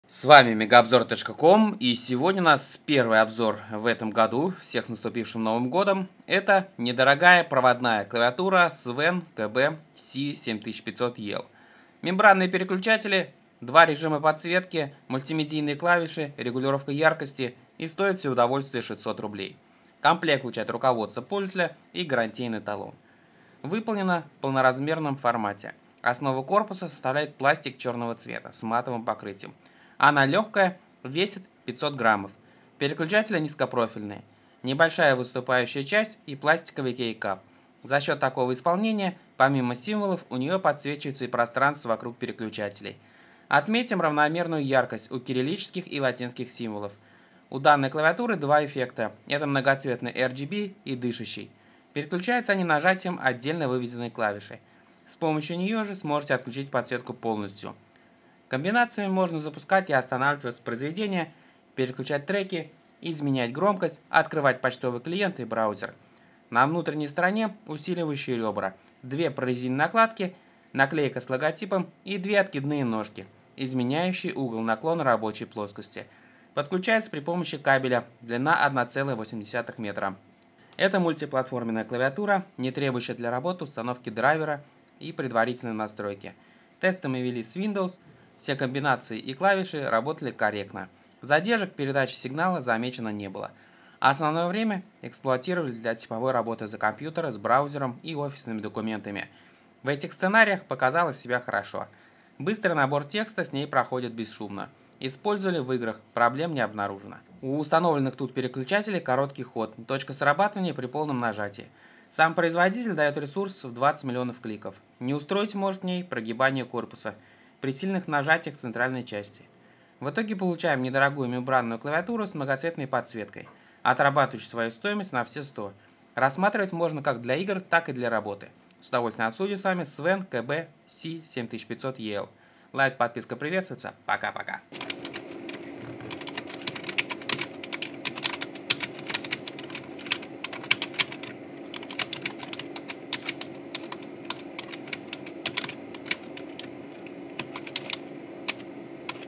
SVEN KB-C7500EL обзор. Недорогая мембранная клавиатура с подсветкой
SVEN KB-C7500EL – недорогая мембранная клавиатура с многоцветной подсветкой, отрабатывающая свою стоимость на все 100%. Рассматривать ее можно как для игр, так и работы.